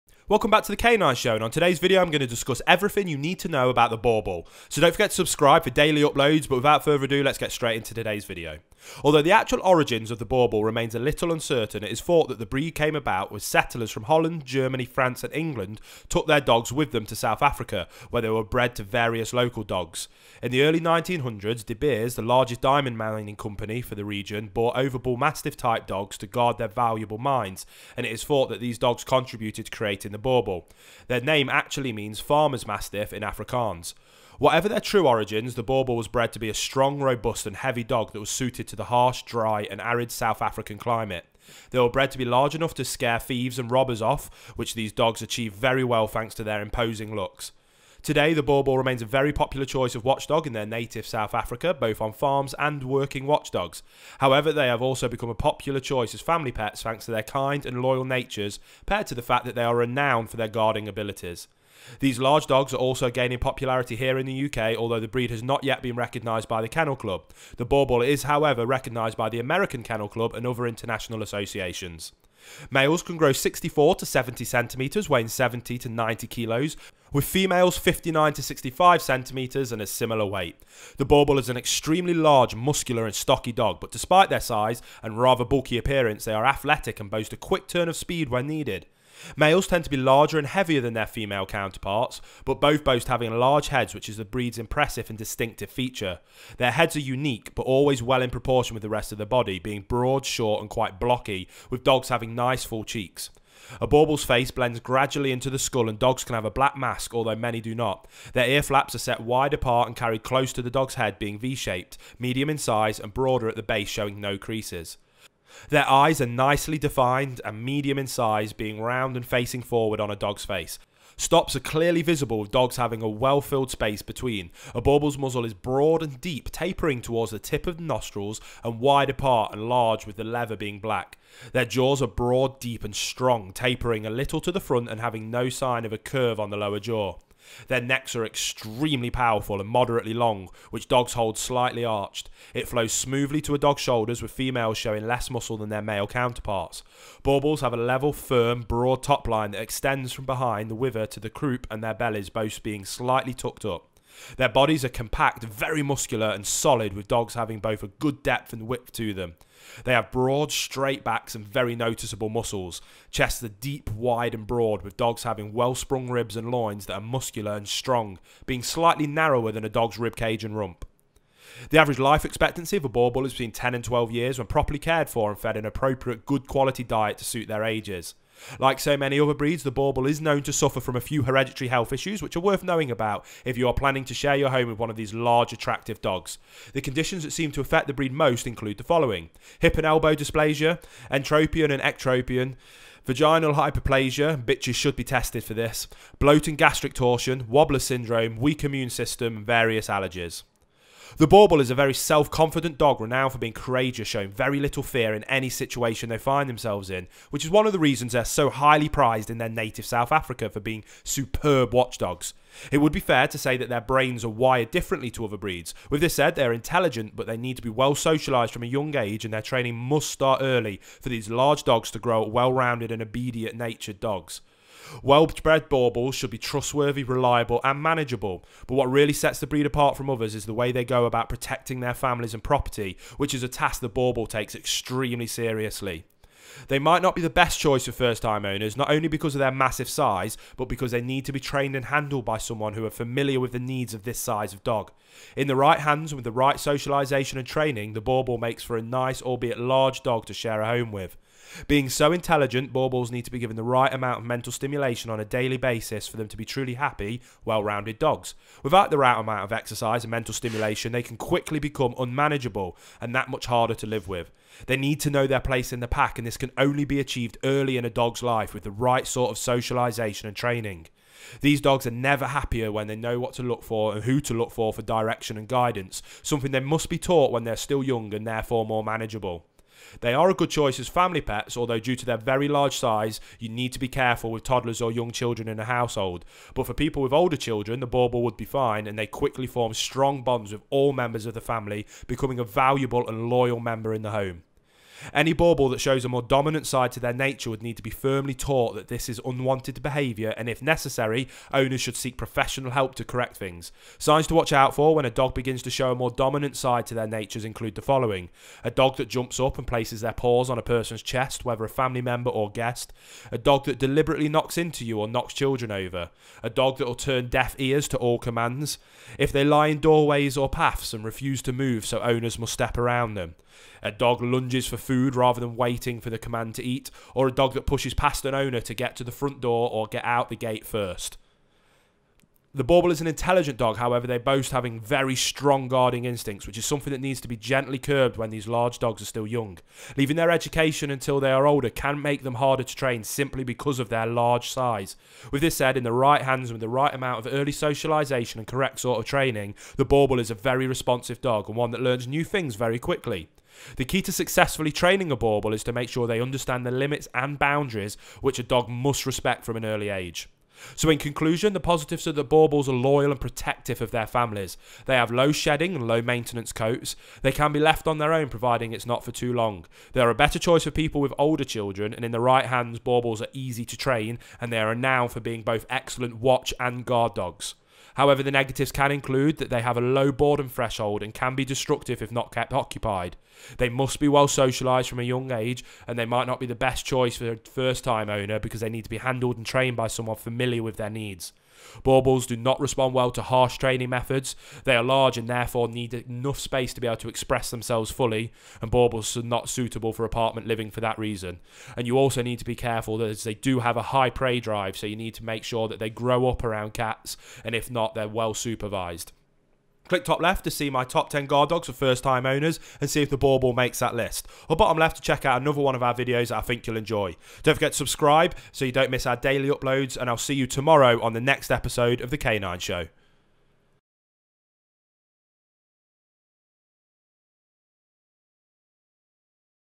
BOERBOEL 101! Everything You Need To Know About Owning A BoerBoel Puppy
BOERBOEL 101! Are you thinking of getting a new BoerBoel or just interested in learning more about BoerBoels? Have you ever wondered if BoerBoels are good with children? or, Is the BoerBoel a good family pet? Then lets get into the video and find out!